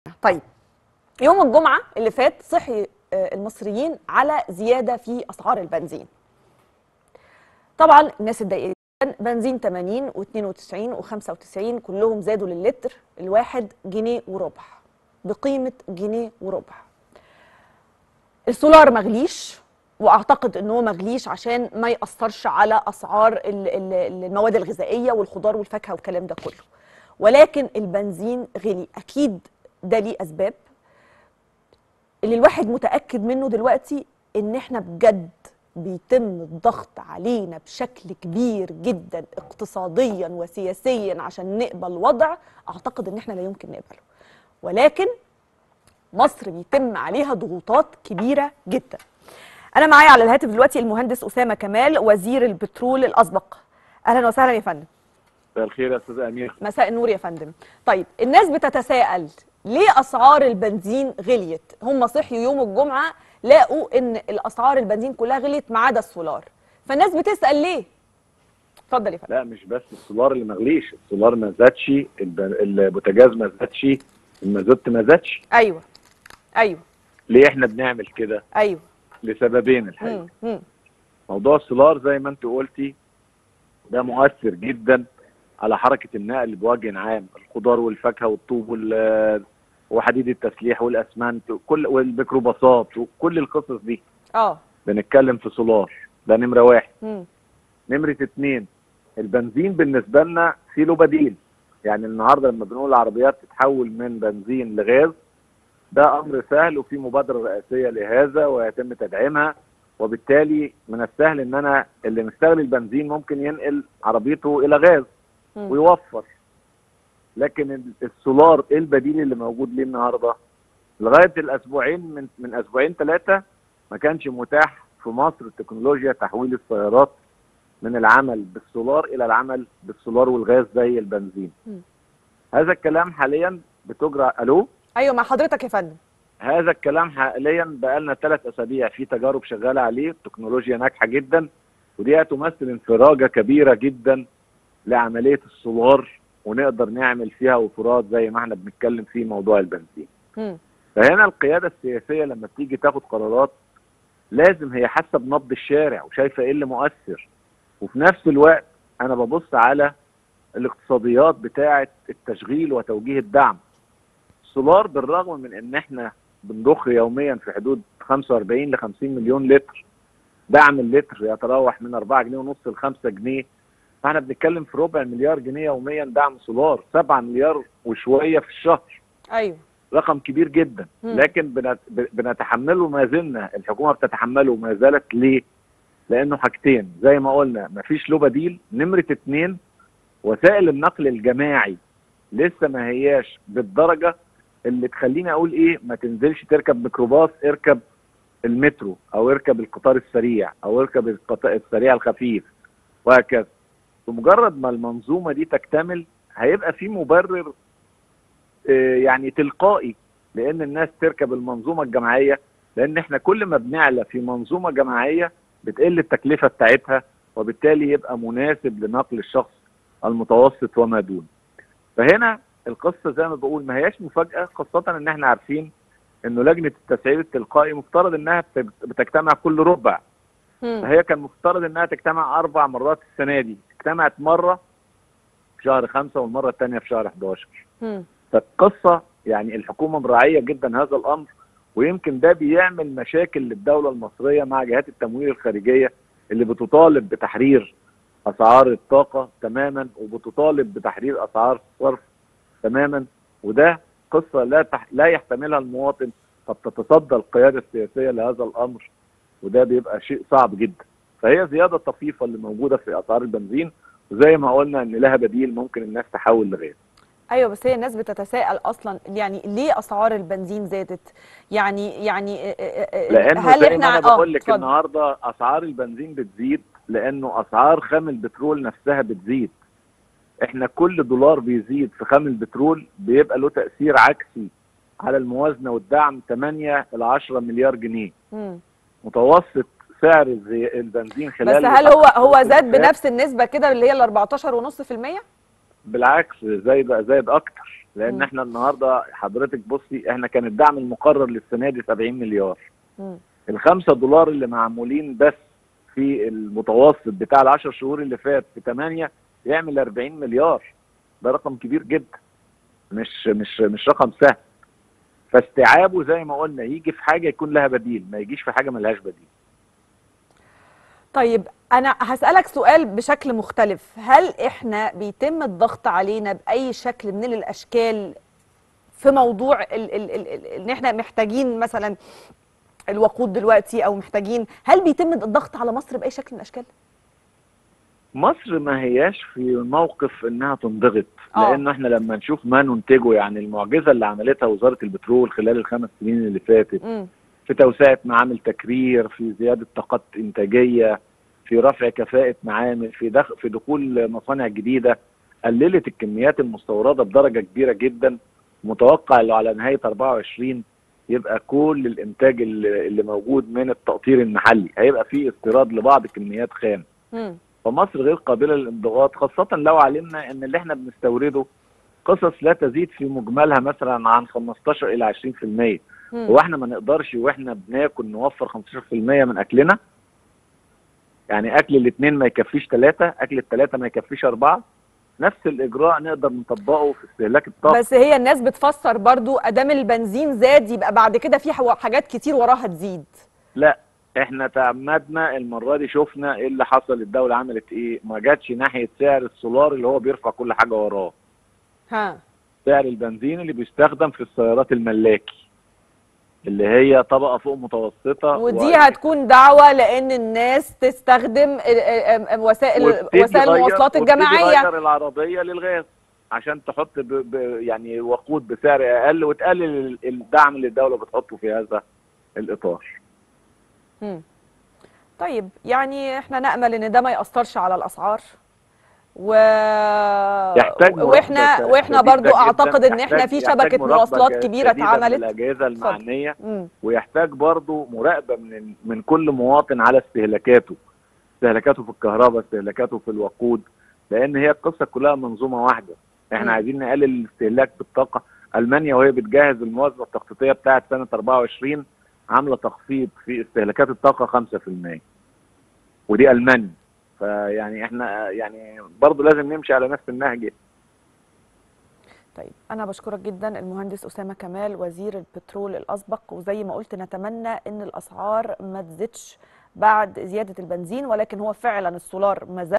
أميرة بدر تنفـ🤬ـعل على الهواء من صدمة أسعار البنزين.. وزير البترول الأسبق يرد: كل واحد يظبط استهلاكه
أميرة بدر تنفـ🤬ـعل على الهواء من صـ*ـدمة أسعار البنزين.. وزير البترول الأسبق يرد: كل واحد يظبط استهلاكه!!